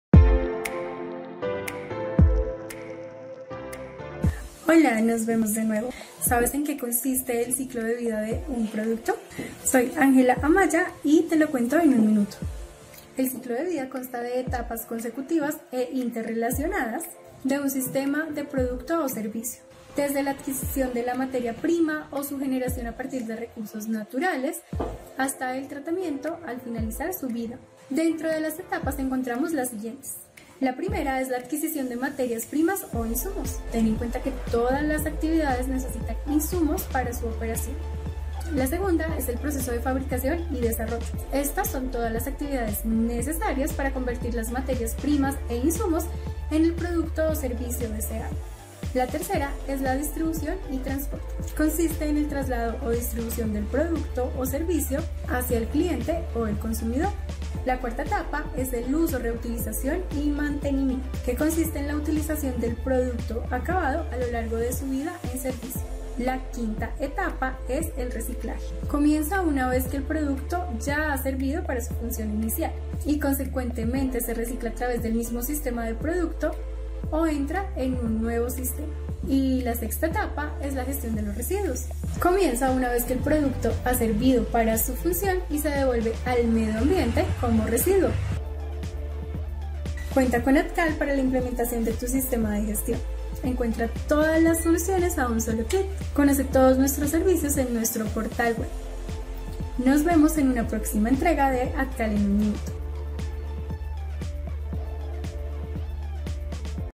ETAPAS DEL CICLO DE VIDA DE UN PRODUCTO | En un minuto
El ciclo de vida consta de las etapas consecutivas e interrelacionadas de un sistema de producto o servicio, desde la adquisición de materia prima o su generación a partir de recursos naturales, hasta el tratamiento al finalizar su vida. En un minuto te contamos cuales son.

Solicita la matriz de riesgo ajustada a las necesidades de tu empresa, contáctanos